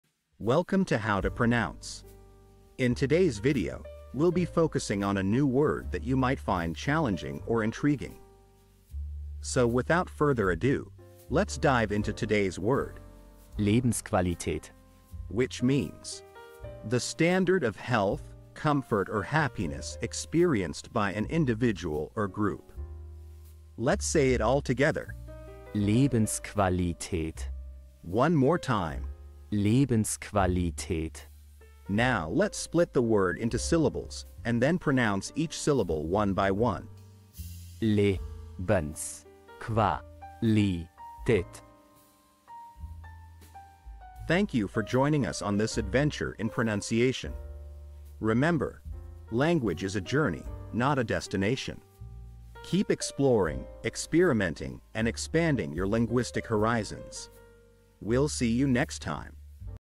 How to pronounce Lebensqualität
🌍 Learn a new language now Master the pronunciation of 'Lebensqualität' Lebensqualität 📖 - which means : The standard of health, comfort or happiness experienced by an individual or group 📝 - with this quick and clear tutorial by How To Pronounce!

In this video 📽️, we'll guide you through the correct pronunciation of 'Lebensqualität' in German, using native speaker examples 🗣️👂 and easy-to-follow phonetic breakdowns 🎵. Whether you're learning German for travel ✈️, business 💼, or just for fun 🎉, our guide will help you perfect your pronunciation in no time ⏰.